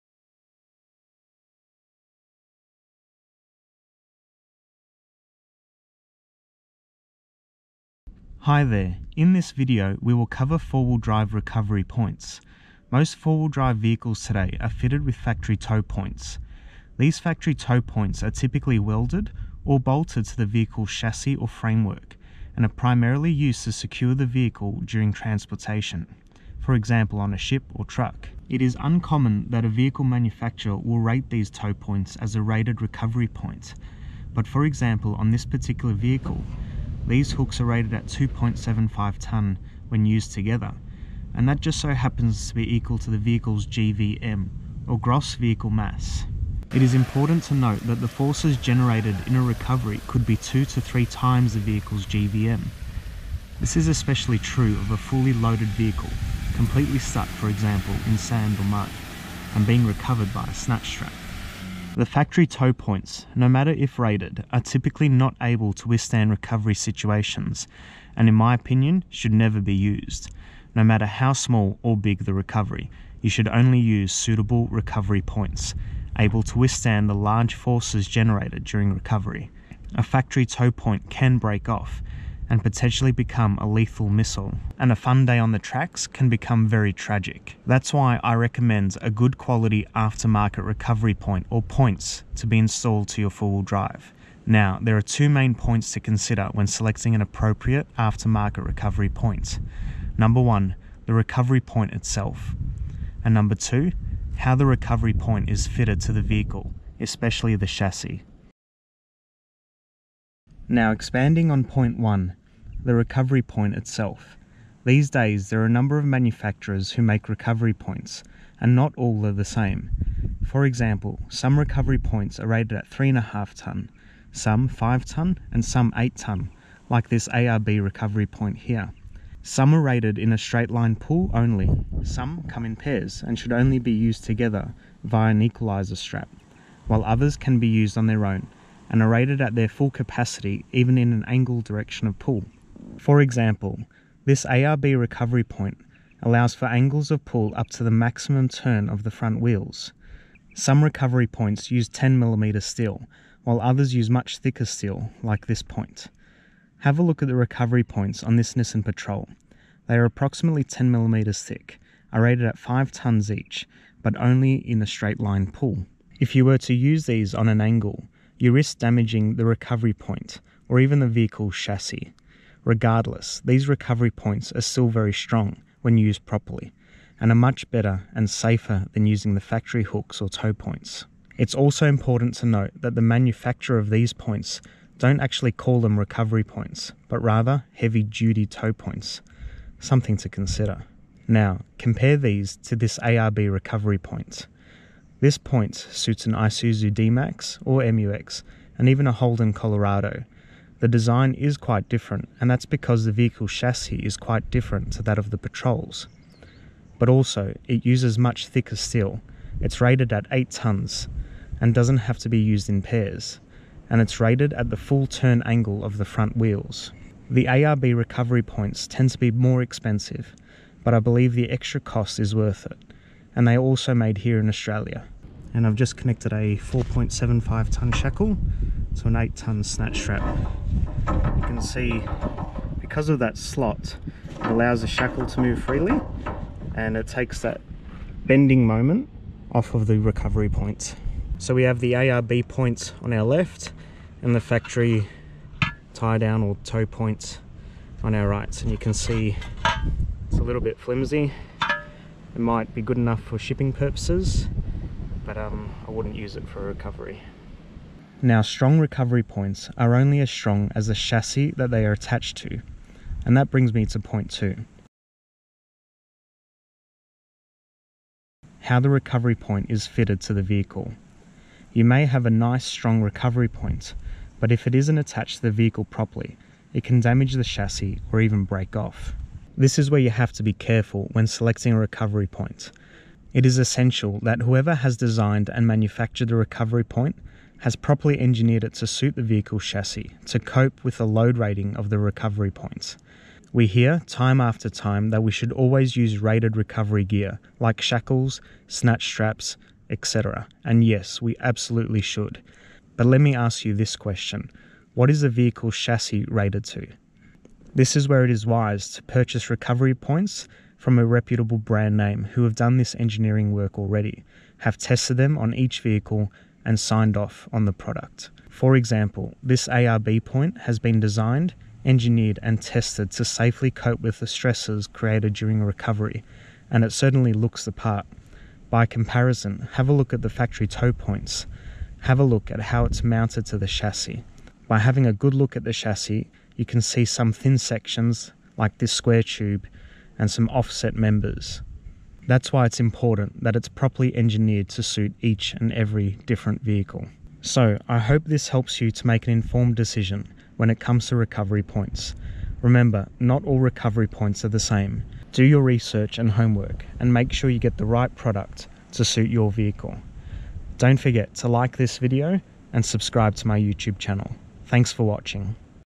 4WD Vehicle Rated Recovery Points - 2 Important Considerations TOW POINTS vs. RATED RECOVERY POINTS!
In the market for some aftermarket recovery points for your 4x4 off-road vehicle? Does your vehicle come equipped with factory 'recovery', 'tow' or 'tie-down' points? Are they suitable for recovery situations?

There are two very important considerations when it comes to purchasing and installing off-road (4x4) vehicle recovery points. Not all 'recovery' points are the same, not all are equal...

In this video, we will cover these two important factors when it comes to recovery points:

1. The recovery point itself and
2. The vehicle's chassis

Some recovery situations can generate forces up to 2 to 3 times the vehicle's Gross Vehicle Mass (GVM). These forces are transmitted through the recovery gear (eg. bow shackles, snatch straps etc.) to the vehicle's recovery point(s) and then through to the vehicle's chassis. Whilst rated recovery gear is essential, what is the vehicle's chassis rated to? A properly engineered, tested and approved recovery point will allow the safe transmission of forces generated from the recovery through to the recovery point and ultimately the chassis. Therefore, a properly engineered recovery point considers not only the recovery point but also how it attaches to the chassis and ensures it won't damage the chassis at the full load rating of the recovery point.

Remember, a strong recovery point is only as weak as the chassis it is attached to...